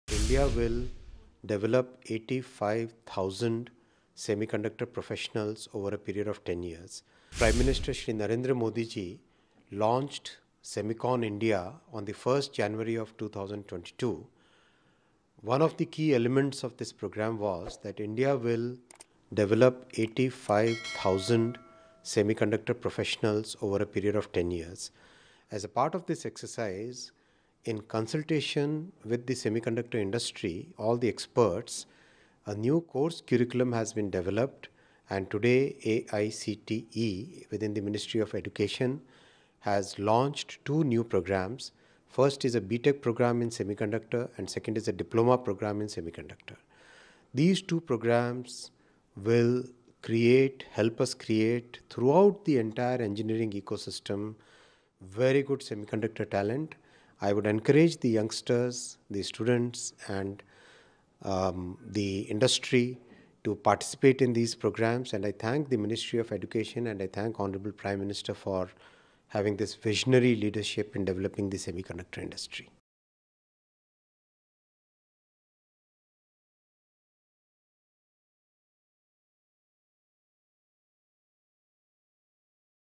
“India to develop...” IT Minister Ashwini Vaishnaw’s big announcement on Semiconductors
Emphasising on Prime Minister Narendra Modi’s dream of making India a self-reliant nation in terms of semiconductors production, Union Minister for Electronic and IT Ashwini Vaishnaw on February 18 announced that India will develop 85,000 semiconductor professionals over 10 years.